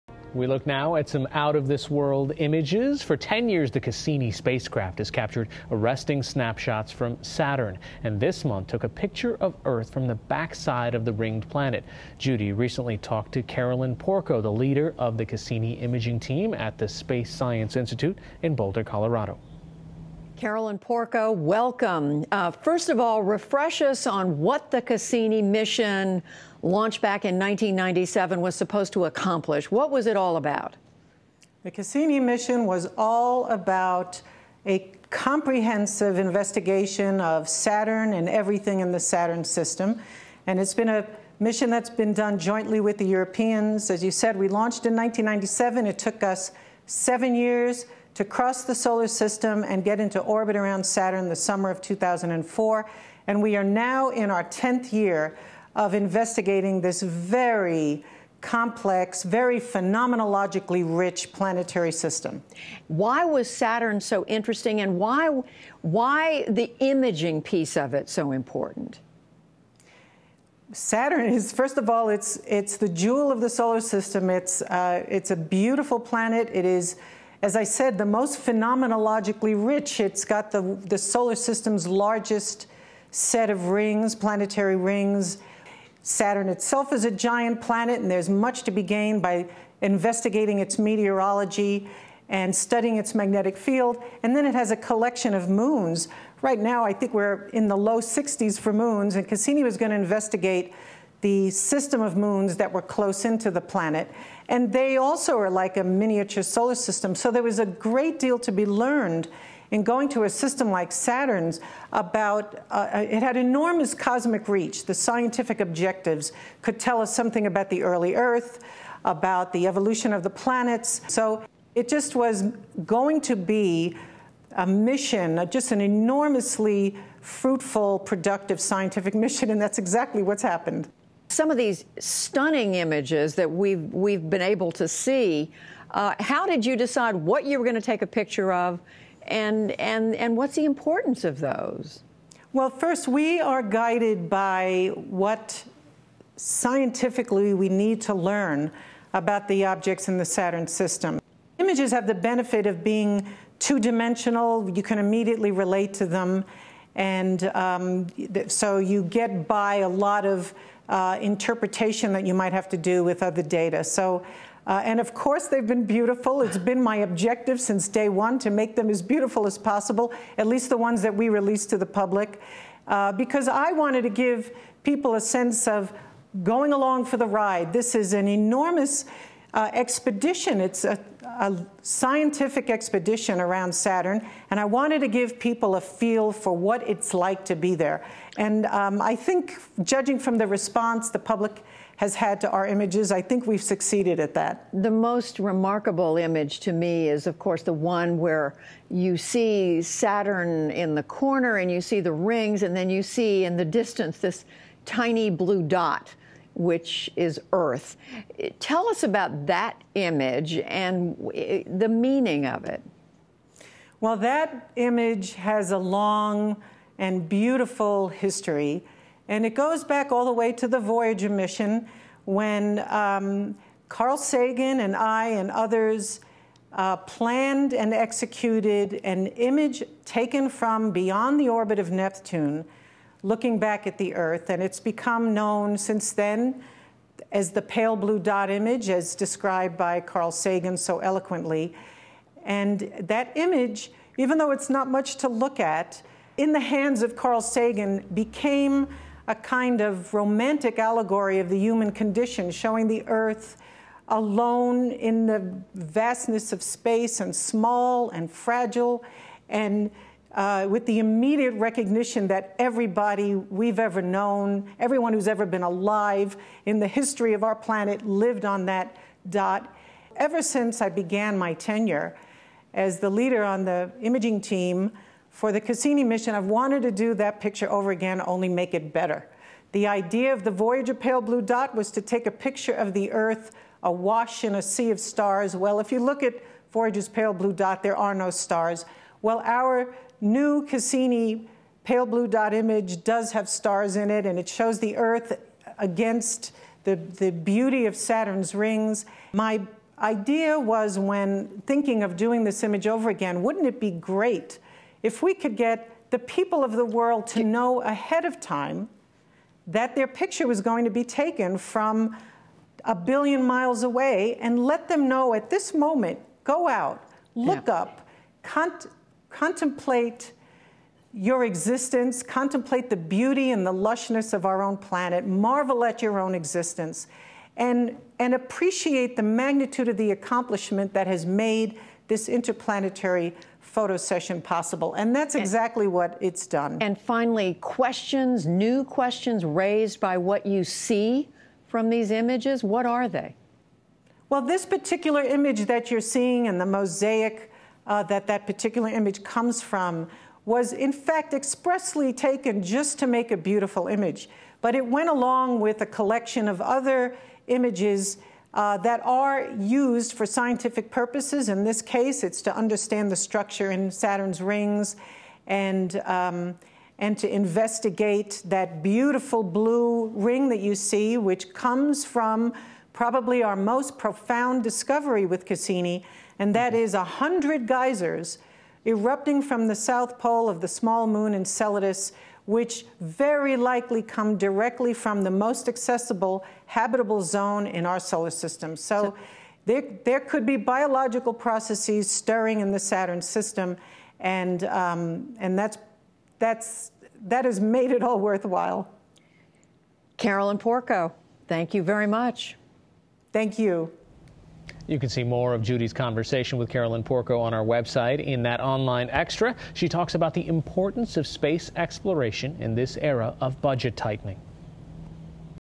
Cassini cosmic photos bring the world 'along for the ride'
The Cassini spacecraft has been capturing snapshots from Saturn for the past 10 years. Carolyn Porco of the Space Science Institute talks to Judy Woodruff about capturing and sharing images of the "jewel of the solar system" and discoveries made about the planet's meteorology and moons.